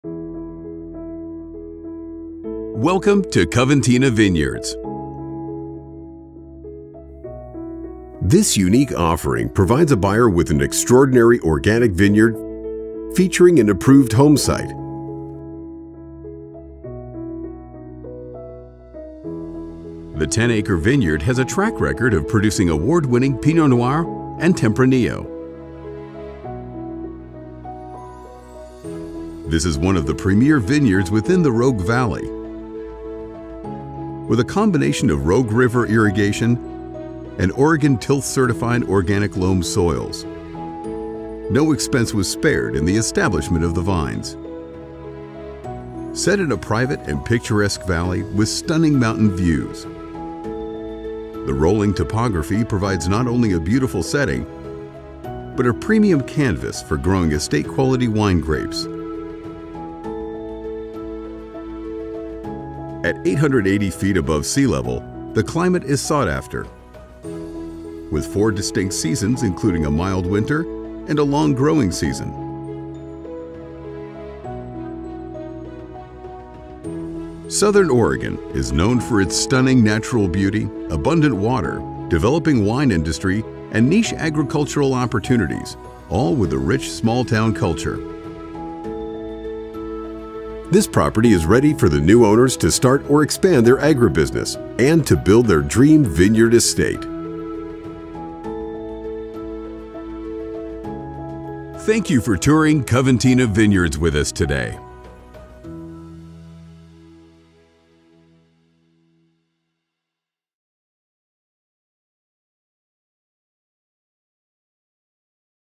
Coventina Vineyard | Award Winning Vineyard For Sale | MOPG x LandLeader
An Award Winning Southern Oregon Vineyard For Sale
This unique offering provides a buyer with an award-winning organic Southern Oregon vineyard for sale with both home site approval and strong river water rights. This vineyard is one of the most premier vineyard installations within the Rogue Valley, boasting a combination of great infrastructure, highly productive soils, and modern irrigation distribution. This Southern Oregon Vineyard for sale is a fantastic opportunity for a rural estate vineyard with perfect Rogue River proximity!

It’s the Climate! | Southern Oregon Vineyard For Sale
Located in the beautiful and temperate Rogue Valley of Southwest Oregon, Coventina Vineyards is situated on 10.40 acres with +/-8.08 net acres of on-site vineyard. In addition to this acreage, there are also +/- 2.00 net acres of vineyard that has a long-term lease (rights to lease included in this offering). This brings the total net acreage for this offering to +/-10.08 net acres. This Southern Oregon vineyard for sale is currently planted in Pinot Noir and Tempranillo with a density of 908 vines/acre (8×6 spacing).

High Quality Soil | Southern Oregon Vineyard For Sale
The vineyard benefits from Oregon Tilth Organic Certification and high quality loam soils. At 880 feet above sea level, the climate is highly sought after with four distinct seasons, including a mild winter and a long growing season.

Rogue River Access Nearby | Southern Oregon Vineyard For Sale
The vineyard’s proximity to the Rogue River adds significant appeal, offering scenic beauty and numerous recreational activities. The river is perfect for fishing, kayaking, rafting, and paddleboarding, with rapids ranging from gentle to challenging. Hiking and wildlife-watching along the river’s lush trails provide breathtaking views and diverse wildlife. Additionally, riverfront parks and campgrounds are ideal for picnicking, camping, and family outings, allowing full immersion in the natural beauty and recreational opportunities of the Rogue River.

This blend of a premium organic vineyard and access to exceptional recreation makes Coventina Vineyards a truly unique and desirable property, ideal for those seeking both agricultural pursuits and a vibrant outdoor lifestyle.

Southern Oregon is known for its mediterranean climate, with four distinct seasons and a mild winter, the region is rich in culture and outdoor adventure with renowned attractions such as Jacksonville’s Britt music festival, Ashland’s Oregon Shakespeare Festival, the Rogue River, the Oregon coast, great restaurants and numerous local breweries and wineries including the famed Applegate Valley Wine Trail.  The Rogue Valley and Applegate Valley AVA’s are a burgeoning scene for the wine industry with a desirable growing season and suitable water sources!